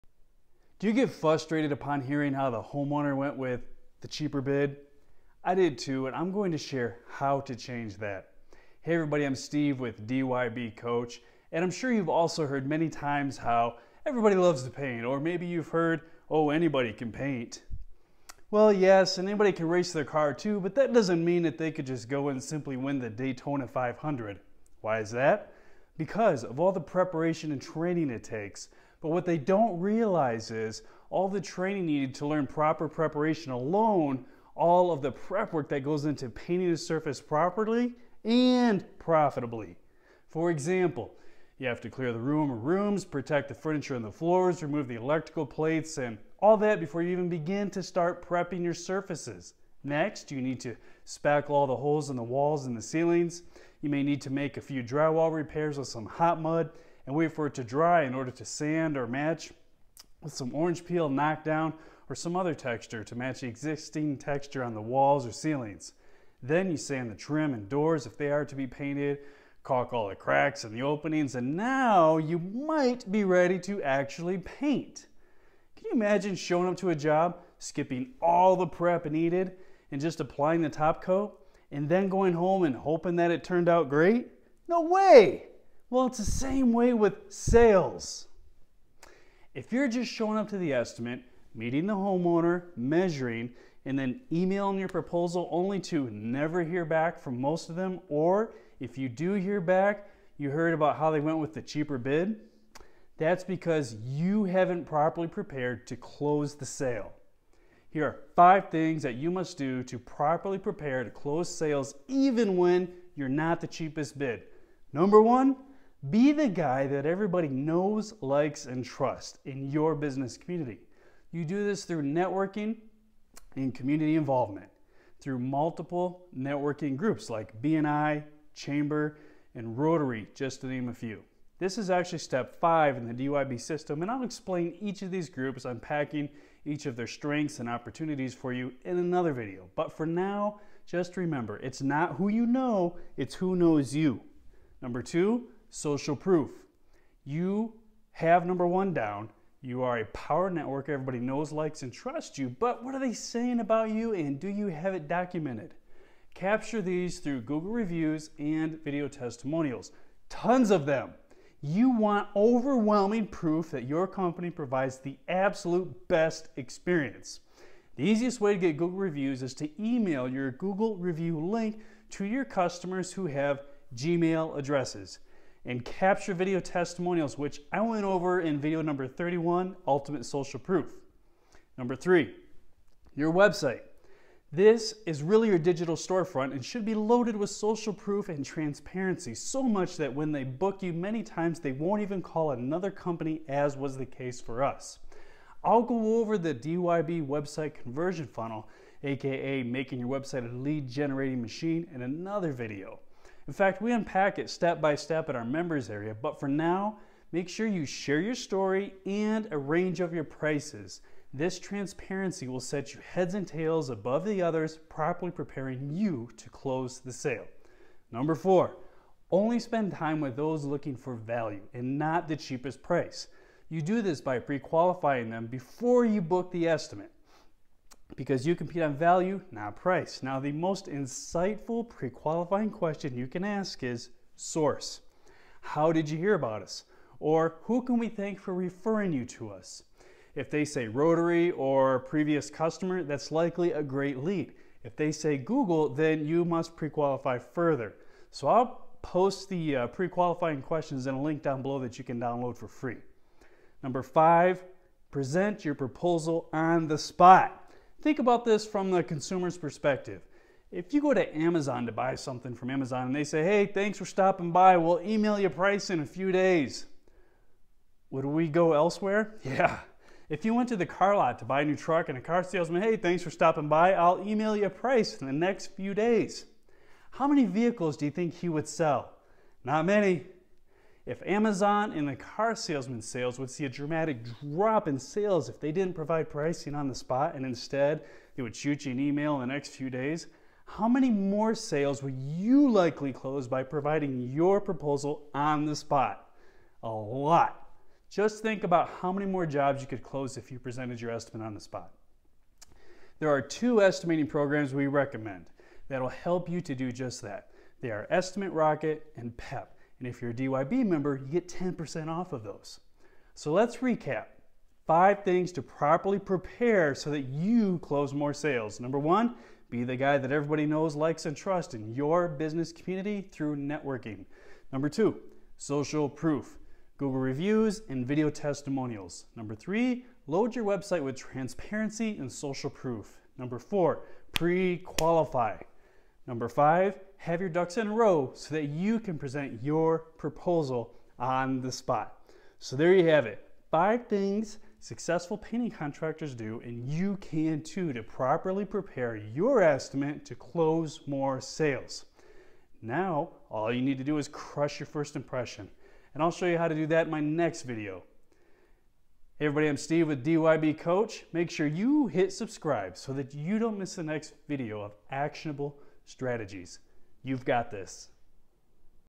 How To Properly Prepare To Close Sales
Download This Proven DYB Prequalifying Process

and You'll Learn.
If they are a good fit - what questions to ask in order to find out without offense?

If they are not a good fit - how do you proceed to refer them away and make them happy at the same time?

Applications (apps) to use while on the phone in order to streamline the process of finding out if they are your ideal client?